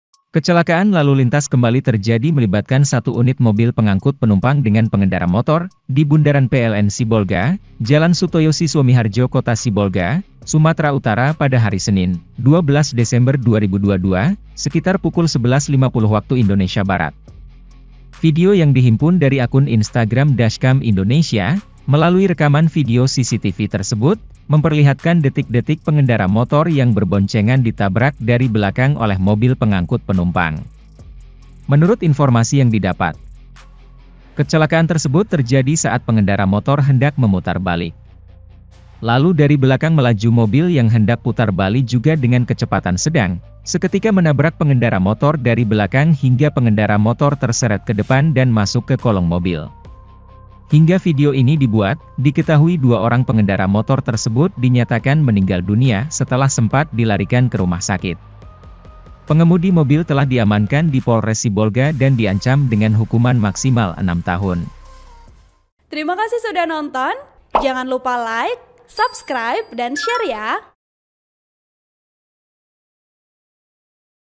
GAK PAKE REM ! Sebuah Mobil Pengangkut Penumpang Menabrak Pemotor Hingga Terseret dan Masuk Kolong
VP : Bimo Bagaswara

TRIBUNJATIM.COM - Kecelakaan lalu lintas kembali terjadi melibatkan satu unit mobil pengangkut penumpang dengan pengendara motor di Bundaran PLN Sibolga, Jalan Sutoyo Siswomiharjo Kota Sibolga, Sumatera Utara pada hari Senin (12/12/2022) Sekitar pukul 11.50 WIB.

Video yang dihimpun dari akun Instagram @dashcamindonesia melalui rekaman video CCTV tersebut memperlihatkan detik - detik pengendara motor yang berboncengan ditabrak dari belakang oleh mobil pengangkut penumpang.

Menurut informasi yang didapat, kecelakaan tersebut terjadi saat pengendara motor hendak memutar balik. Lalu dari belakang melaju mobil yang hendak putar bali juga dengan kecepatan sedang, seketika menabrak pengendara motor dari belakang hingga pengendara motor terseret kedepan dan masuk ke kolong mobil.

Hingga video ini dibuat, diketahui dua orang pengendara motor tersebut dinyatakan meninggal dunia setelah sempat dilarikan kerumah sakit. Pengemudi mobil telah diamankan di Polres Sibolga dan diancam dengan hukuman maksimal enam tahun.